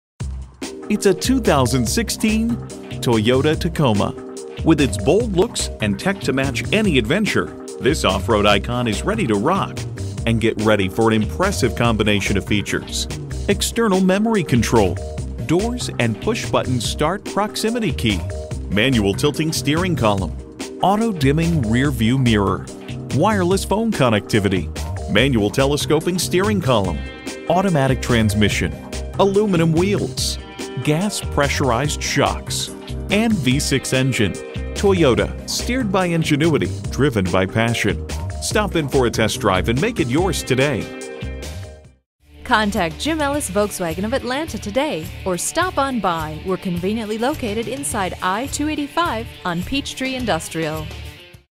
Used 2016 Toyota Tacoma Atlanta, GA KP4968 - SOLD
Year: 2016
Make: Toyota
Model: Tacoma
Trim: TRD Sport
Engine: V6
Transmission: 6-Speed Automatic
Color: Gray
Interior: Graphite
Mileage: 35279
Stock #: KP4968
VIN: 3TMBZ5DN4GM004528
THIS SPECIAL PRICING IS AVAILABLE WITH APPROVED CREDIT. SEE DEALER FOR DETAILS.brbr2016 TOYOTA TACOMA TRD SPORT V6brbr* CLEAN CARFAX, 1-OWNERbr* 3.5L V6 ENGINEbr* 7" ENTUNE PREMIUM TOUCHSCREEN AUDIObr* NAVIGATION & APP SUITEbr* SMART KEY w/PUSH BUTTON STARTbr* BLUETOOTH HANDS-FREE CONNECTIVITYbr* REARVIEW CAMERAbr* L.E.D. DAYTIME RUNNING LAMPSbr* SPORT TUNED SHOCKSbr* 17" ALLOY WHEELSbr* BRAKE ASSIST & SMART STOP TECHNOLOGYbr* CRUISE CONTROLbr* FOG LAMPSbr* LEATHER WRAPPED STEEIRNG WHEEL & SHIFT KNOBbr* SLIDING REAR WINDOWbr* V6 TOW PACKAGEbr* 120v POWER OUTLETbr* DECK RAIL SYSTEMbr* 7-PIN CONNECTOR W/ CONVERTERbr* TIRE PRESSURE MONITORING SYSTEMbr* 17" ALLOY WHEELSbrbrAwards:br* JD Power Initial Quality Study (IQS)br* 2016 KBB.com Best Buy Awards Finalistbr* 2016 KBB.com Best Resale Value AwardsbrbrGive us a call to confirm availability and schedule a hassle free test drive! We are located at 5901 Peachtree Blvd, Atlanta, GA, 30341.br

Address:
5901 Peachtree Industry Boulevard